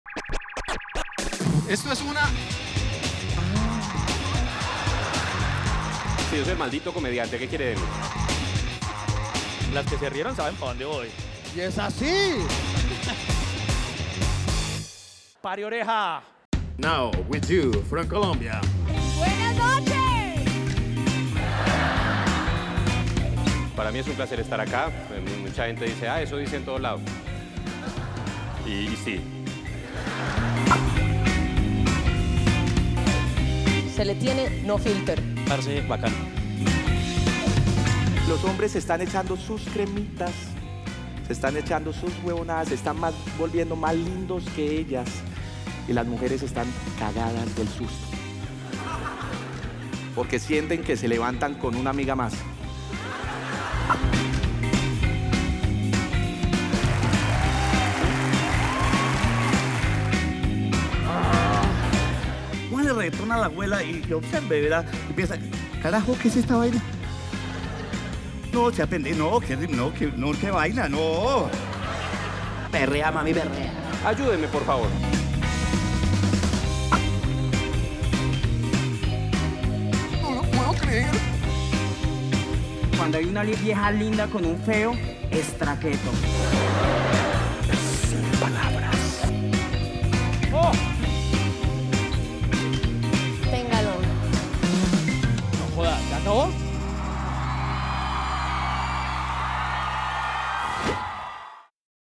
Comediantes de Colombia
Reel con algunos de los mejores exponentes del stand up en Colombia. Edición elaborada para el espacio de Stand Up de Comedy Central. Video subido a red sin propósitos comerciales.